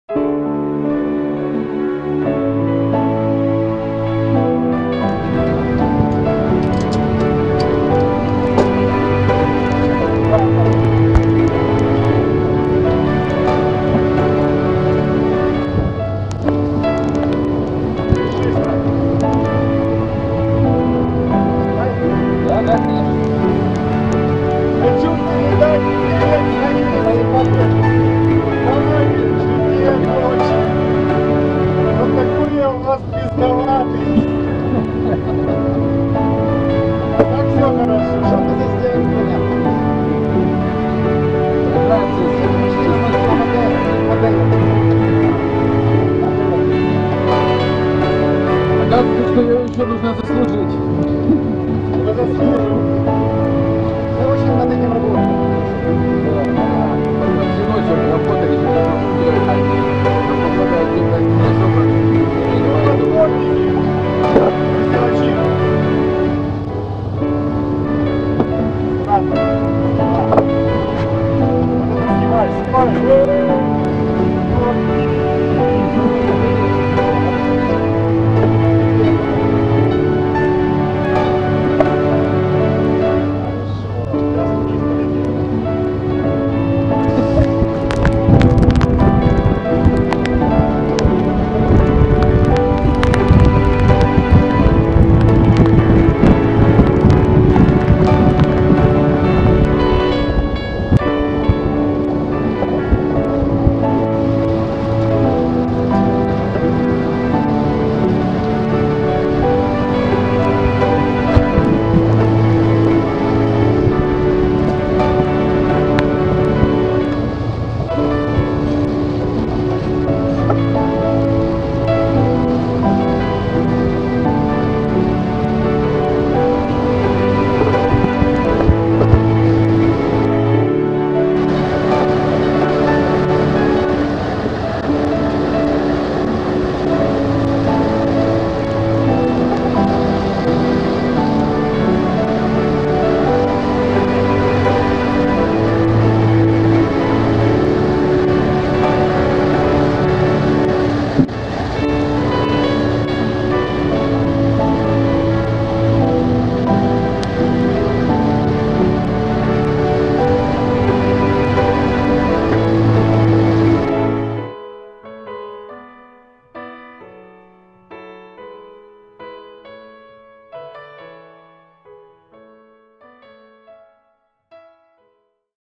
Gibraltar Madeira 4 November 2014
Яхт переход Малага - Гибралтар - Мадейра - Пальма - Гомера - Тенерифа
Штормонуло на сутки где то между Гибралтар - Мадейра
Оператор - Иванчег (айфон шлак))))))
Капитан - Василий
Боцман - в роли Боцмана )))
остальная команда - играет в прятки )))))